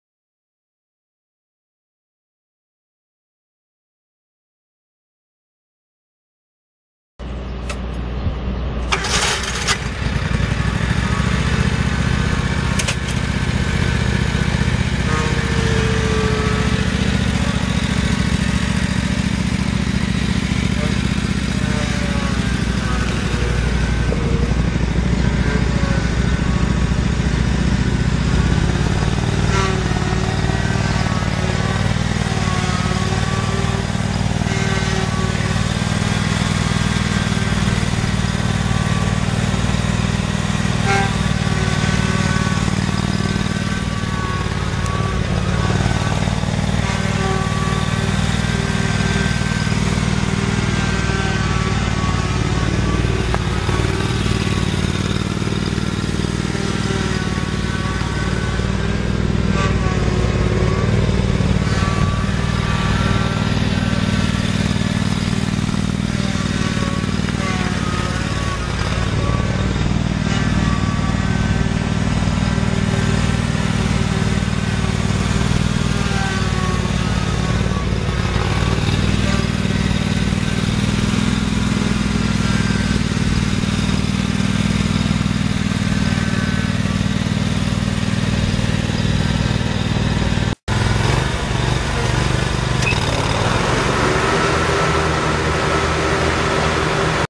2002 John Deere 757 ZTR mower for sale | sold at auction December 7, 2011
SOLD! Ag Dealer Inventory Hotlist Auction

2002 John Deere 757 ZTR mower
574 hours on meter
7 Iron commercial 60" deck
Kawasaki FH721D V-twin gas engine
25 HP
Hydrostatic transmission
24x12-12 tires
Serial TC0757B011082
Stock# 59570

It was located in Clinton, OK, and we sold it to the highest bidder for $3,630.